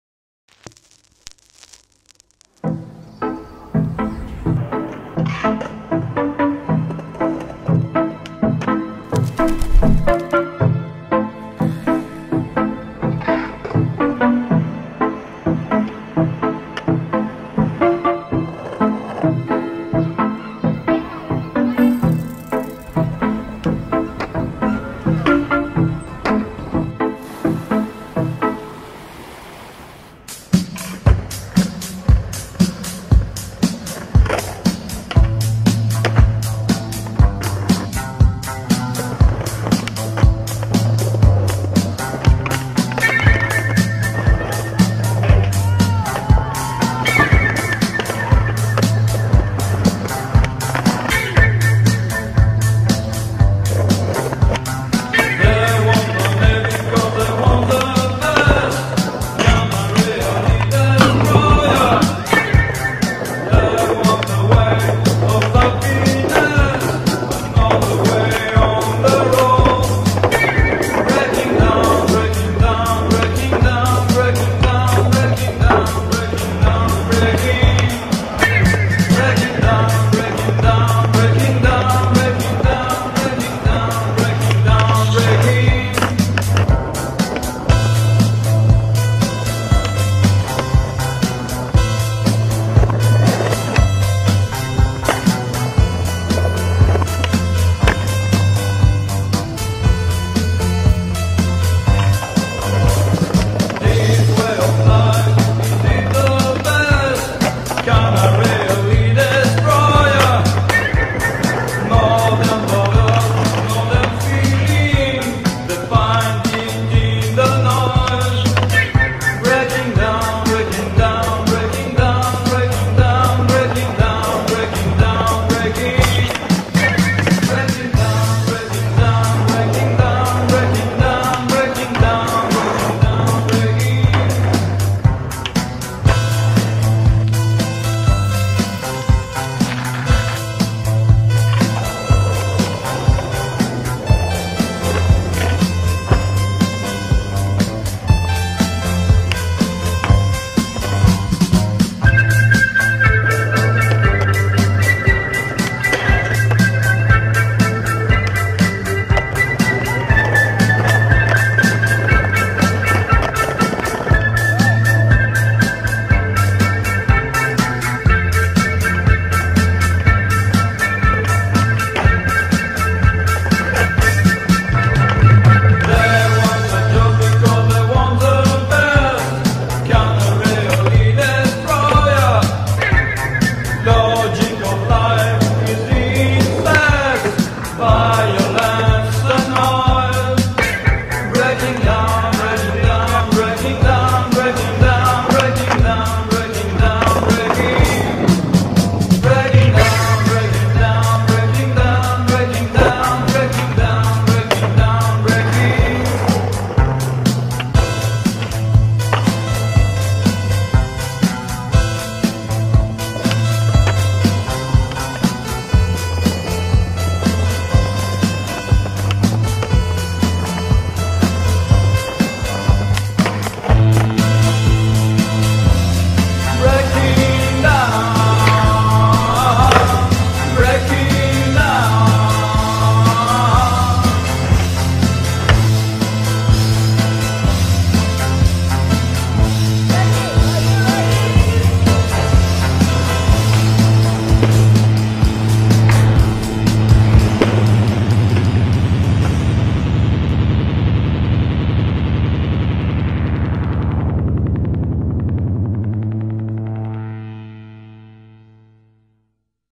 VALENCIA /////// SKATEDELUXE
Winter in Central and Northern Europe can be tough. To break out of this misery, the skatedeluxe crew, along with Denny Pham, Pol Catena, Matt Debauche, Sascha Scharf, Ike Fromme and Douwe Macare, took off to the Spanish Mediterranean coast.

A week in Valencia, the capital of the autonomous Valencian community. Plenty of time to escape the winter blues. In the summery temperatures, the crew explored everything they could get their hands and wheels on, in one of the most beautiful cities in Spain.